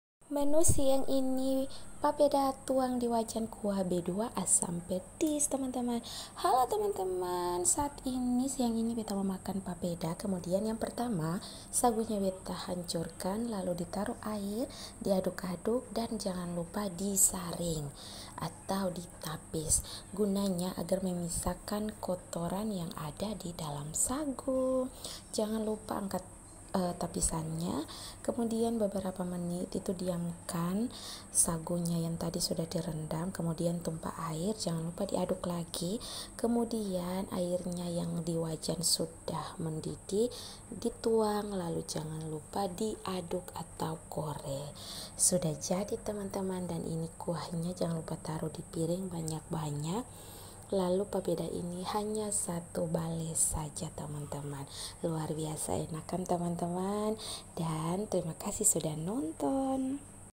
Papeda Kuah B2 Asam Pedis Biking Nambah 3 Bale
Hallo teman2 kali ini menu siang papeda kuah b2 asam pedis